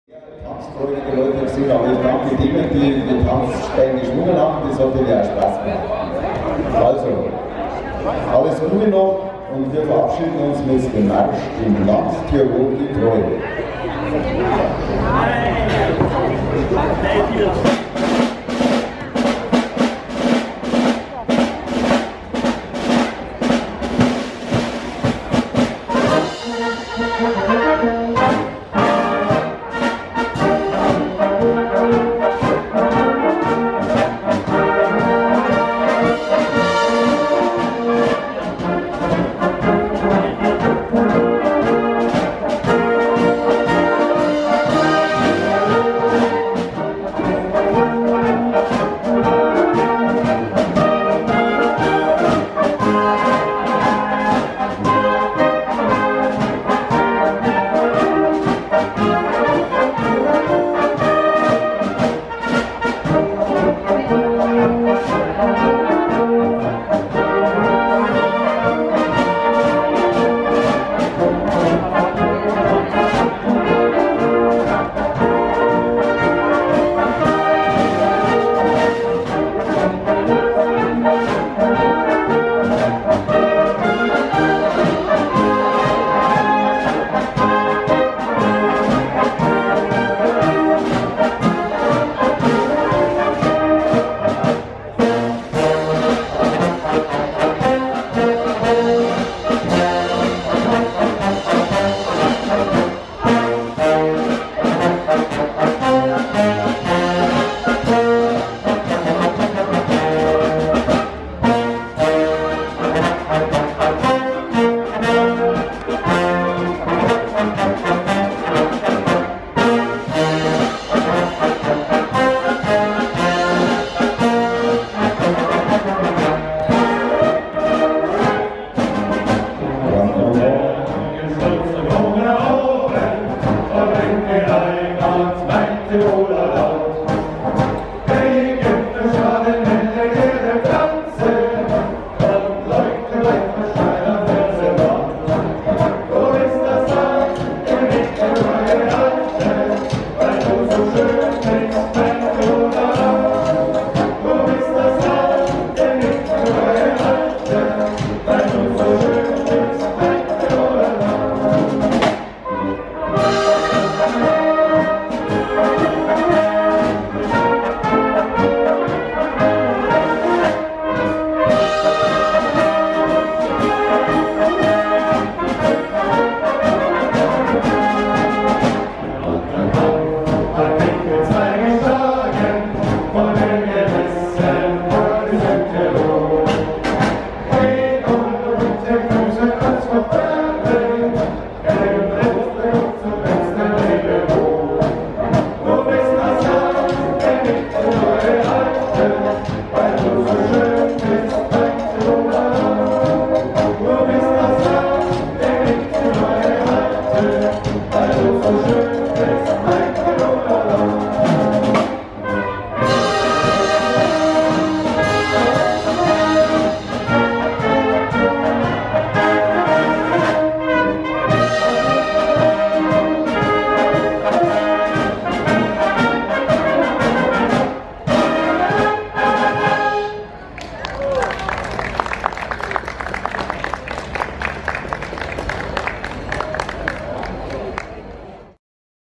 114_Dult_2017, 3/3; Stadtkapelle-Landshut ...
Dirigent: Bernhard Schuder ... Die 678. Landshuter Bartlmädult (Jahrmarkt) ... Festzelt auf der Grieserwiese ... Landshut (Bavaria) ... 21. August 2017 ...